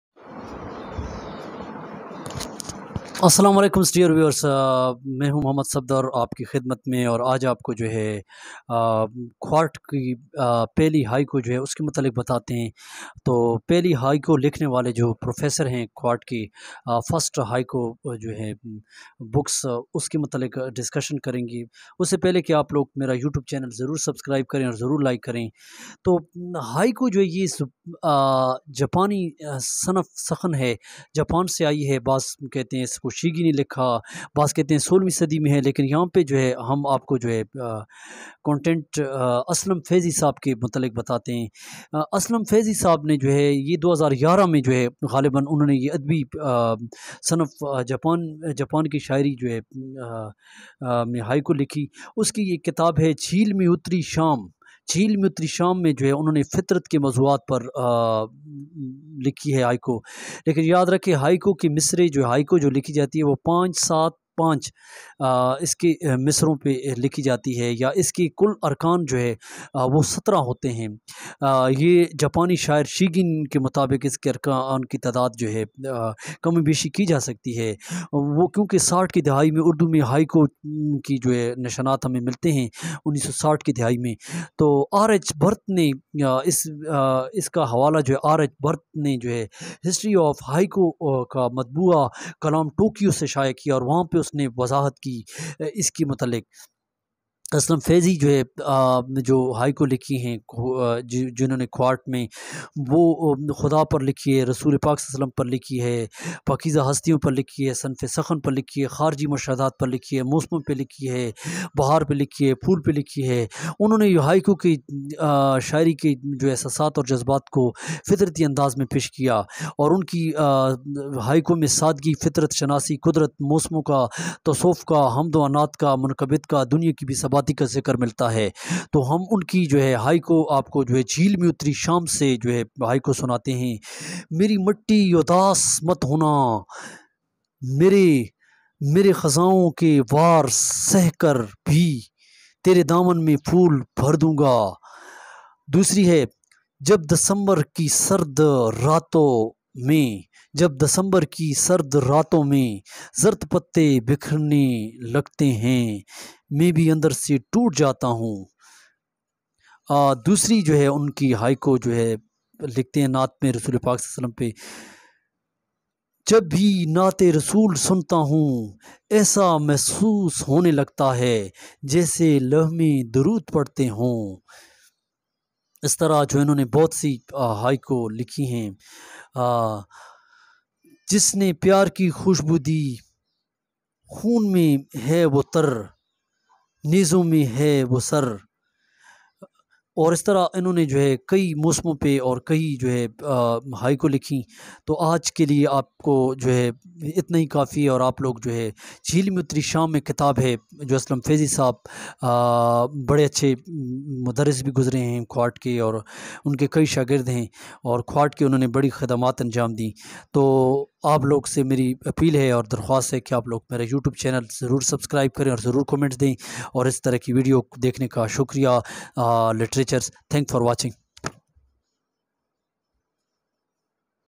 Kohat first hikko کوہاٹ کی پہلی ہائیکو لکھنے والے مصنف ویڈیو دیکھیں
who written squad for first highcourse in high Court comes from Japanese and force awards and what's meanings and their different kind of high Court and Urdu poetry and difference books reference and during the poets and teachings his life and all narrations about the poetry of Urdu and his books writers and what's the he do and what is written about the Urdu poetry